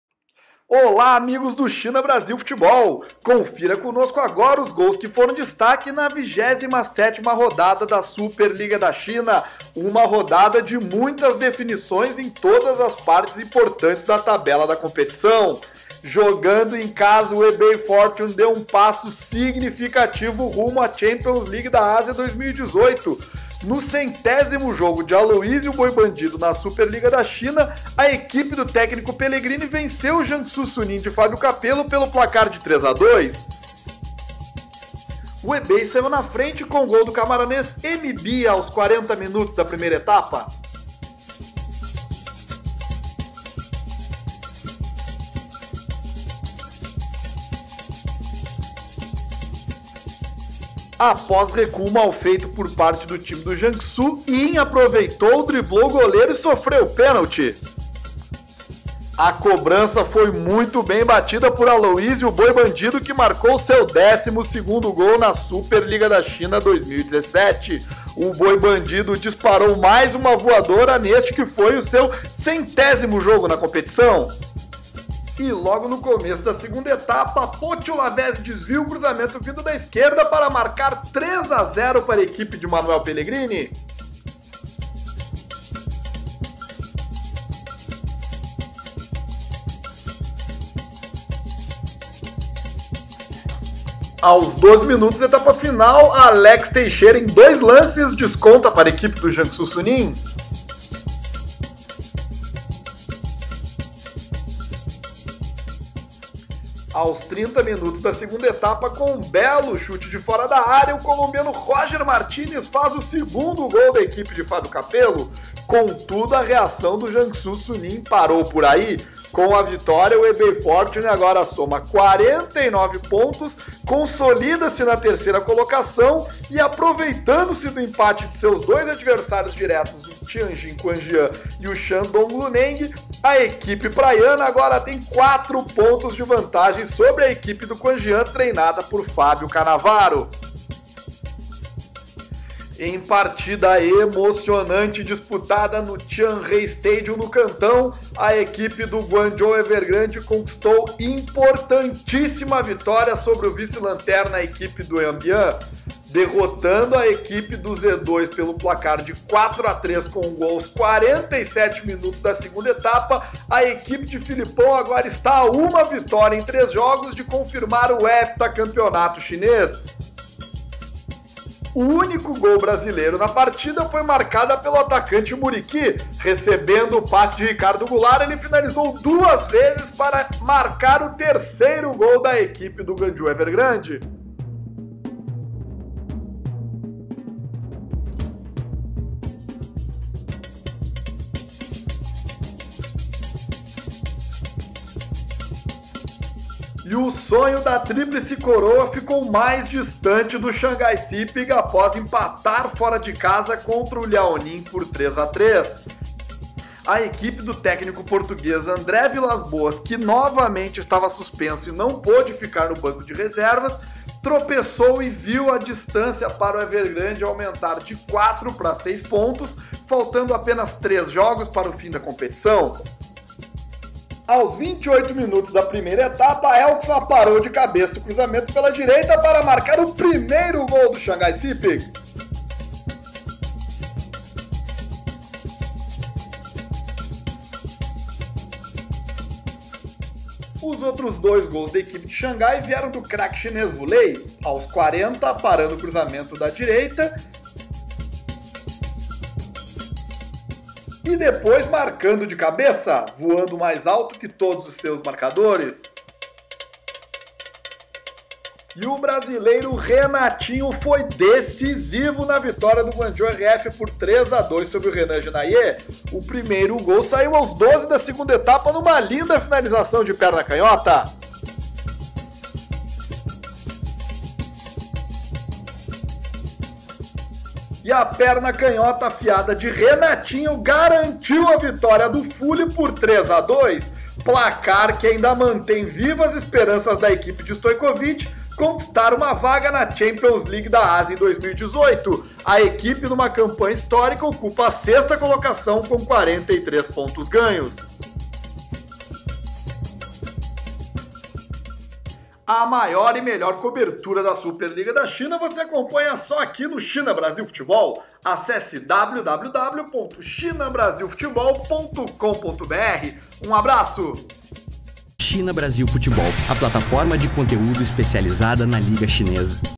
Gols Destaques da 27a Rodada da Super Liga da China 2017
Alô, você!! Voltamos com os gols destaques da 27a Rodada da Super Liga da China. Aloísio Boi Bandido marcou no seu centésimo jogo na Super Liga da China. Trazemos também os gols de Mbia e Lavezzi que decidiram a vitória do Hebei sobre a equipe do Jiangsu Suning, que marcou com Alex Teixeira e Roger Martinez.
Confira também os gols do SIPG com Elkeson e Wu Lei.
O brasileiro Renatinho marcou dois e decidiu a favor do Guangzhou R&F.
Muriqui deixou sua marca na vitória do cada vez mais líder Guangzhou Evergrande.

Acesse para a maior e melhor cobertura do futebol chinês no Brasil!